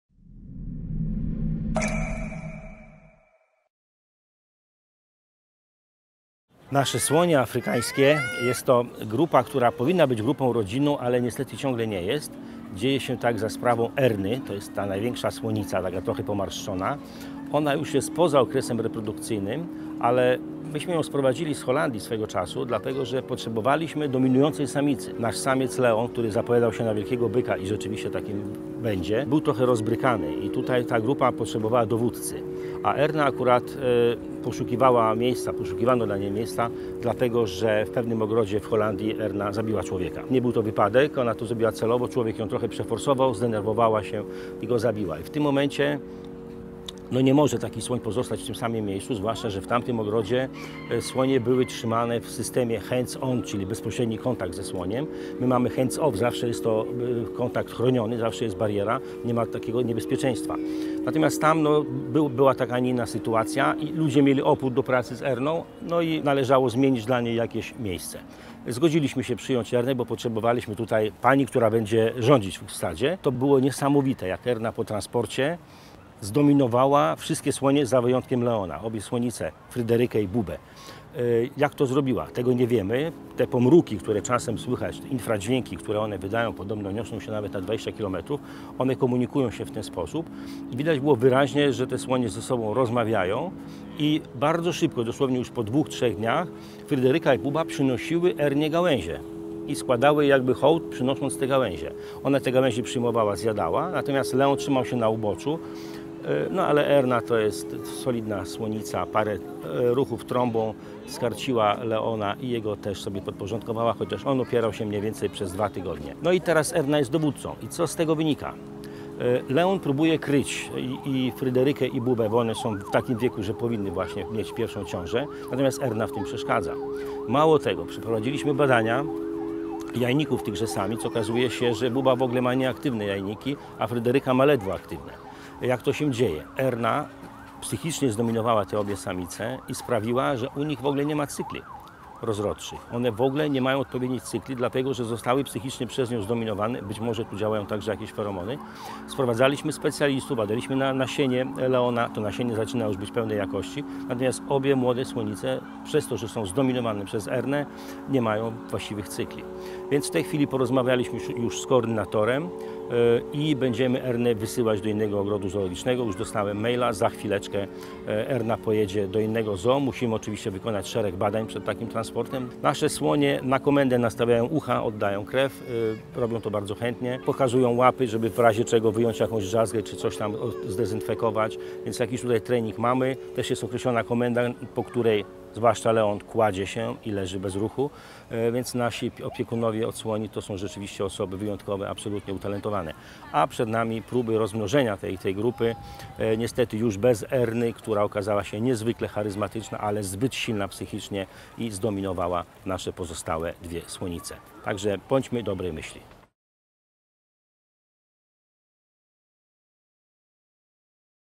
ZOObacz co w Warszawskim ZOO - odc. 2 - Słonie
Bohaterami drugiego odcinka "ZOObacz co w Warszawskim ZOO" są Erna, Leon, Buba i Fryderyka, czyli słonie ze stołecznego zoo.
Czy wiecie, że za przyjazdem Erny do Warszawy kryje się tragiczna 😳 historia?
A czy znacie przykłady niesłychanie silnej 💪 psychiki słonia?
Na pewno zauważyliście, że w naszym stadzie 🐘🐘🐘🐘 nie ma żadnego malucha, dlaczego? 🤔
O tym wszystkim opowiada kolejny odcinek naszego filmowego cyklu "ZOObacz co w Warszawskim ZOO".